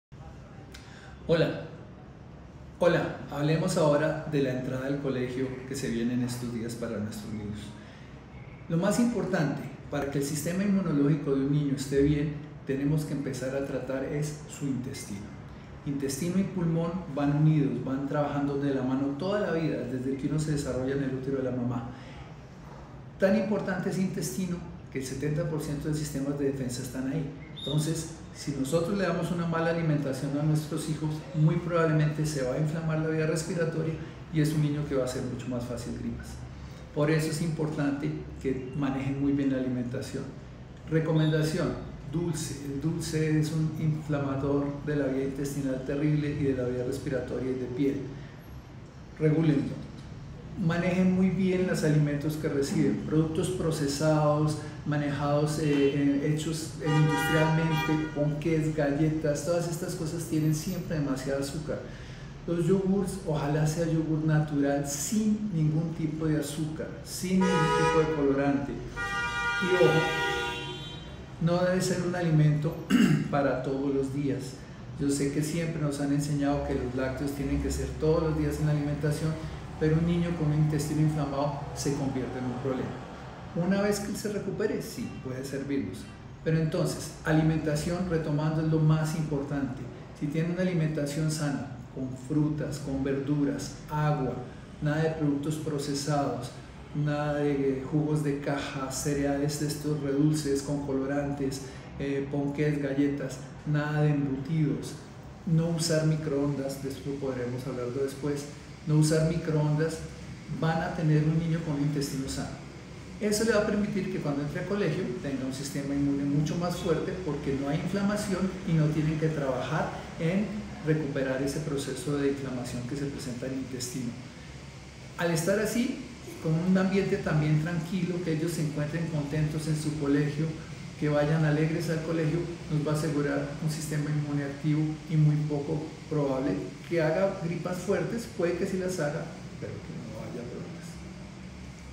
¿Cómo mejorar el sistema inmunológico de los niños para regresar al colegio?
El Dr. Ricardo Gómez,  experto en homeopatía pediátrica, nos acompaña en esta ocasión con consejos muy útiles para mejorar el sistema inmunológico de nuestros hijos antes de regresar al colegio.

El Dr. Gómez nos explica porqué una buena alimentación mantiene altas las defensas y cómo el intestino y los pulmones trabajan de la mano.

¡Somos Bebé a Bordo, queremos crecer junto a ti!